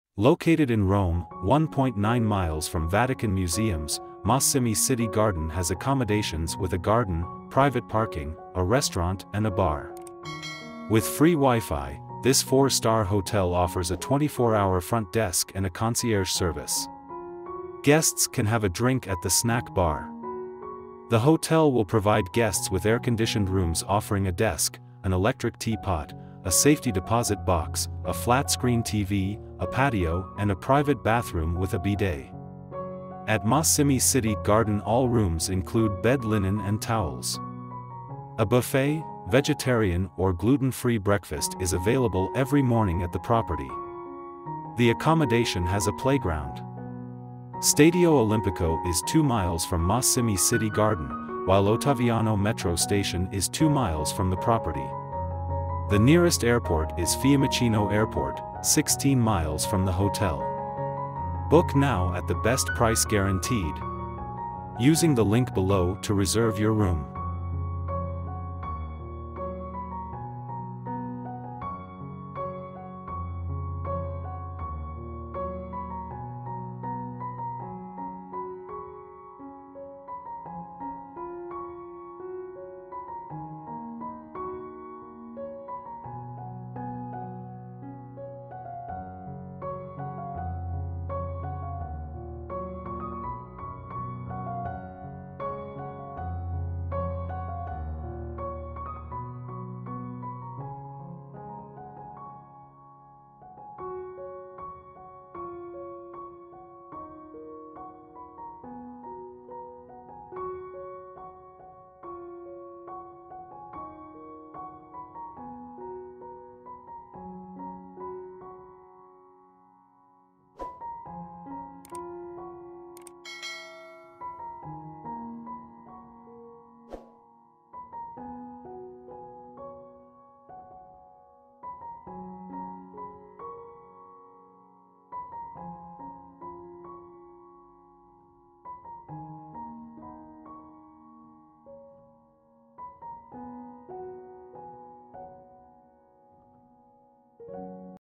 Massimi City Garden
Book now

SUPPORT US
SPECIAL DISCOUNT

Located in Rome, 1.9 miles from Vatican Museums, Massimi City Garden has accommodations with a garden, private parking, a restaurant and a bar. With free WiFi, this 4-star hotel offers a 24-hour front desk and a concierge service. Guests can have a drink at the snack bar.

The hotel will provide guests with air-conditioned rooms offering a desk, an electric tea pot, a safety deposit box, a flat-screen TV, a patio and a private bathroom with a bidet. At Massimi City Garden all rooms include bed linen and towels.

A buffet, vegetarian or gluten-free breakfast is available every morning at the property.

The accommodation has a playground.

Stadio Olimpico is 2 miles from Massimi City Garden, while Ottaviano Metro Station is 2 miles from the property. The nearest airport is Fiumicino Airport, 16 miles from the hotel.